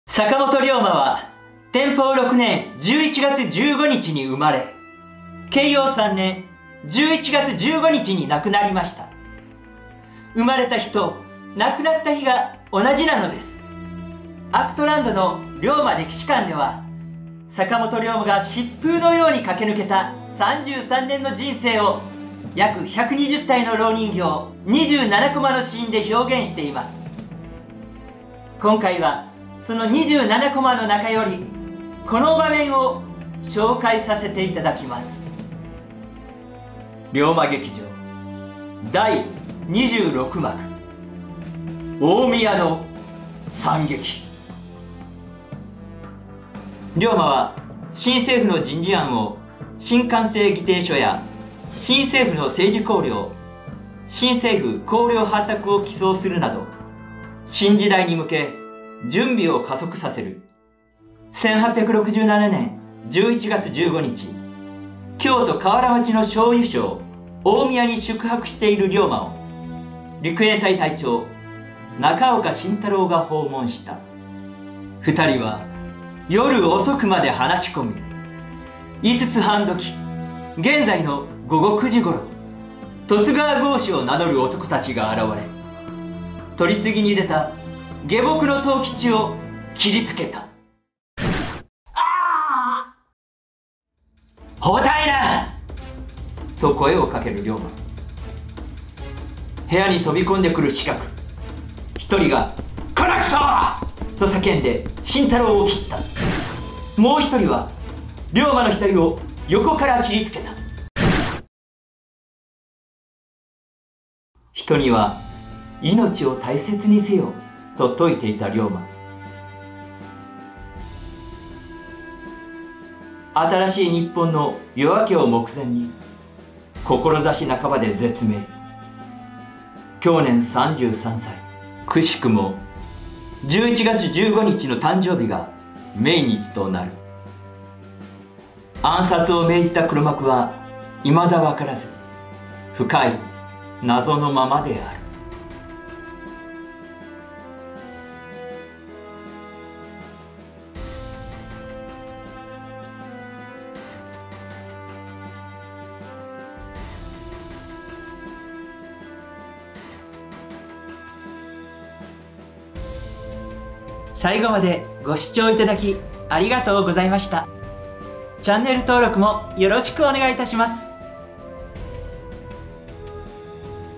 【坂本龍馬】龍馬劇場紹介・近江屋の惨劇【蝋人形】
11月15日は坂本龍馬が生まれ、そして亡くなった日になります。
今回は龍馬歴史館の龍馬劇場第26幕「近江屋の惨劇」この場面がどうのようなシーンなのかご紹介いたします。
蝋人形を始めとした立体的なコマの作りをお楽しみ下さい。
創造広場「アクトランド」（所在地：高知県香南市。高知空港から車で10分）は、芸術・文化・技術の総合テーマパーク！面白い企画や展示でご家族で楽しめるスポットです！